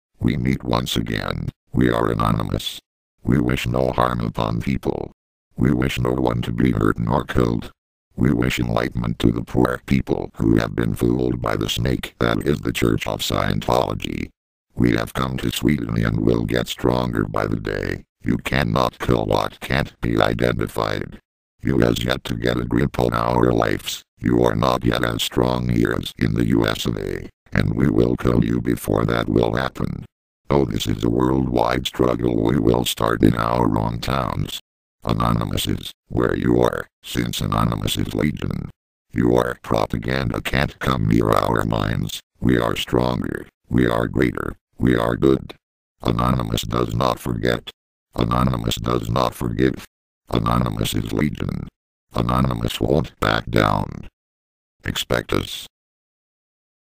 Anonymous in Sweden
This is the first of many calls against scientology